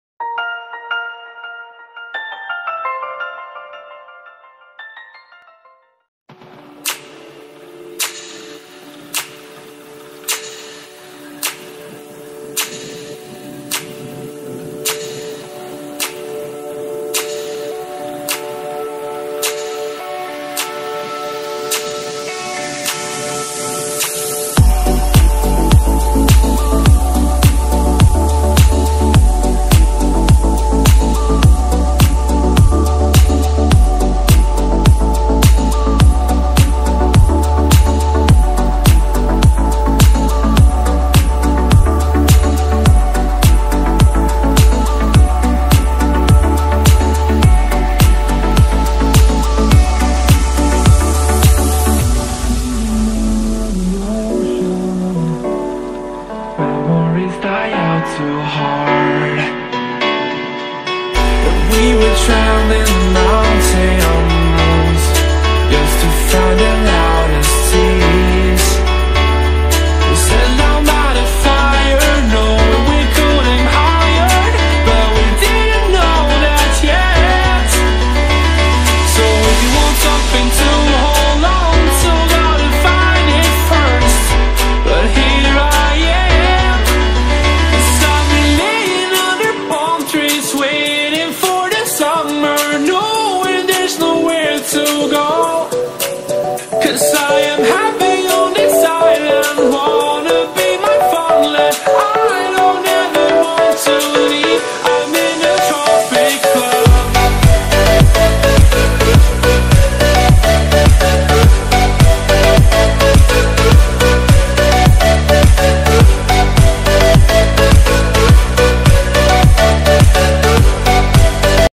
Honda Africa Twin | Victory Red Edition | First Look | Walk Around
Honda Africa Twin - Victory Red Colour - Walk Around

navjivan automobiles pvt.ltd.
d-5,navin flouring compound,opp.safari complex,
udhna bhestan main road,bhestan,surat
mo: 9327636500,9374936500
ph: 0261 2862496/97